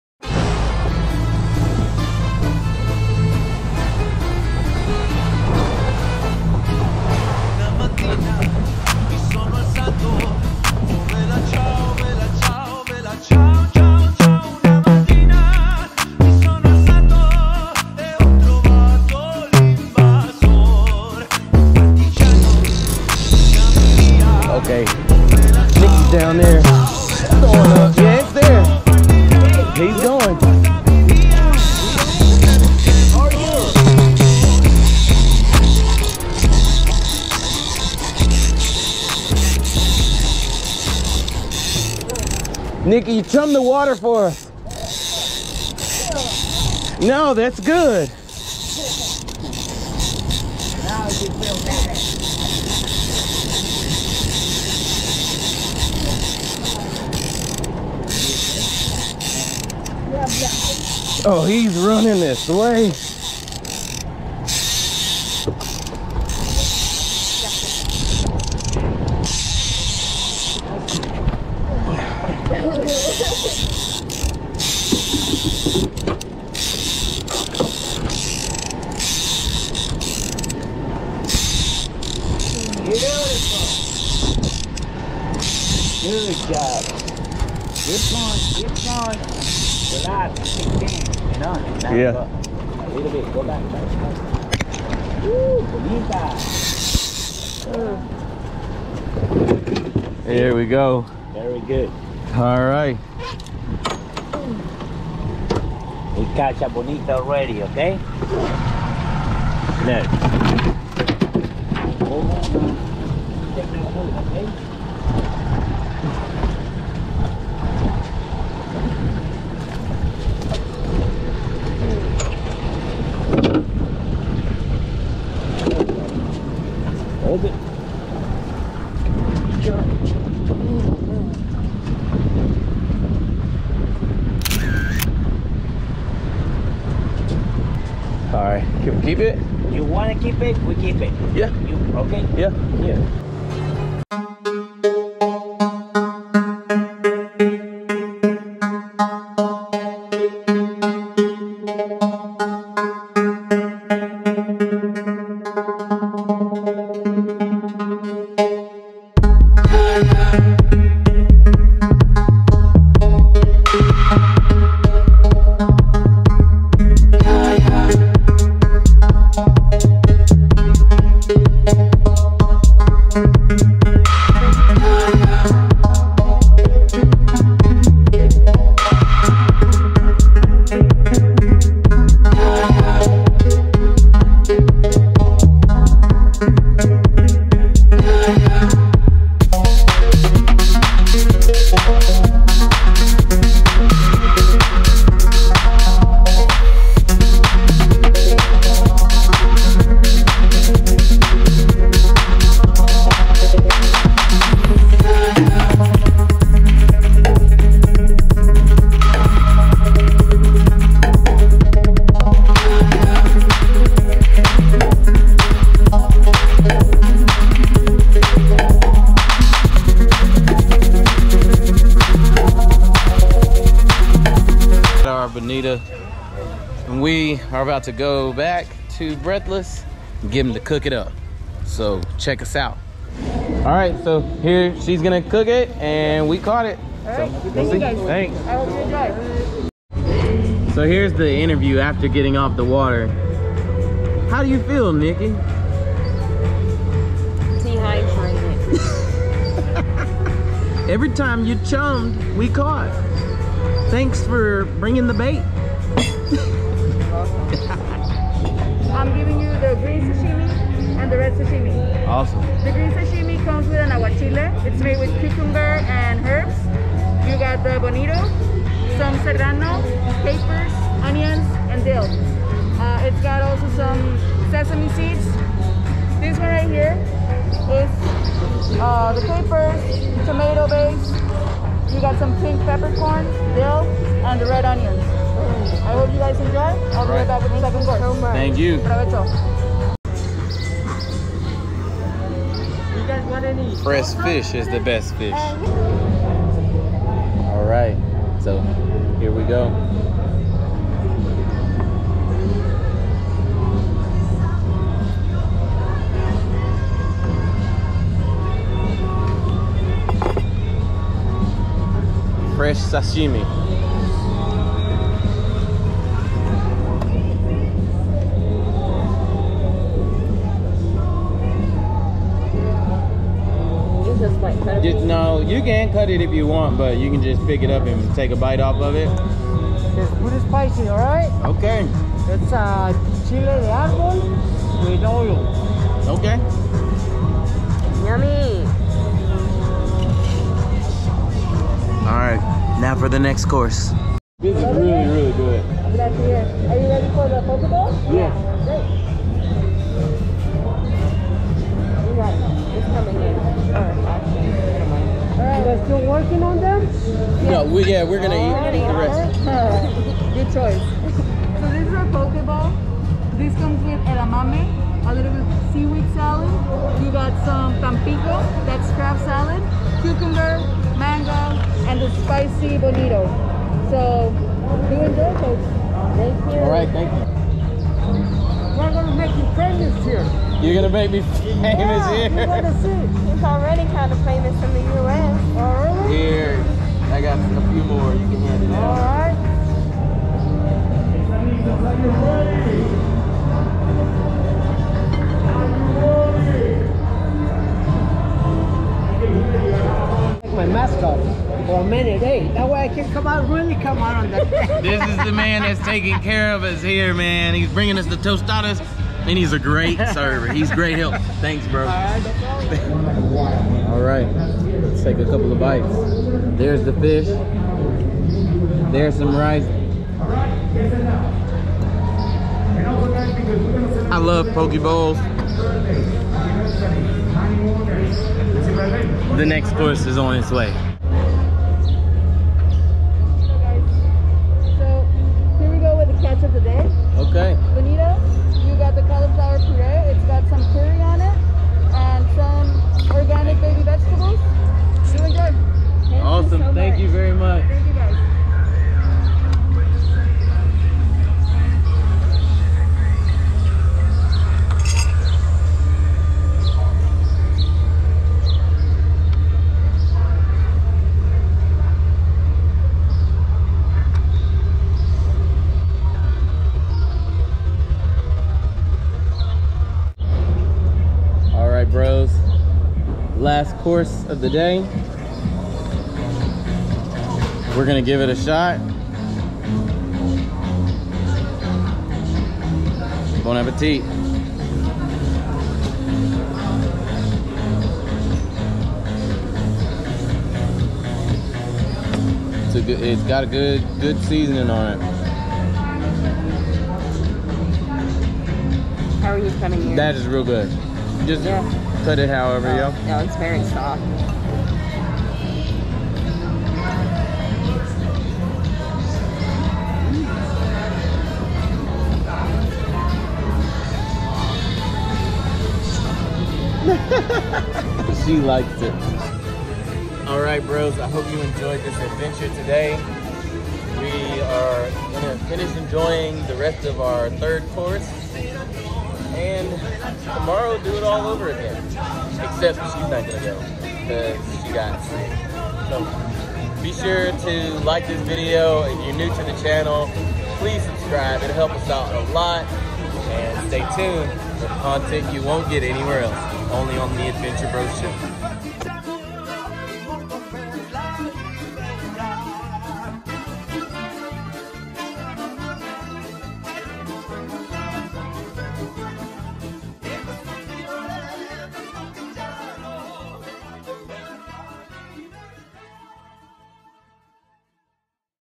THE BEST MEAL IN CABO! Fresh Bonito from SEA to TABLE
First time catching a Bonito. Why not clean it and eat it! We have a chef prepared 3 course meal in Cabo San Lucas. Our trip to Mexico was incredible and this Trash fish turned Gourmet was the most tasty meal we had the entire trip! Some exciting catches on the way!!!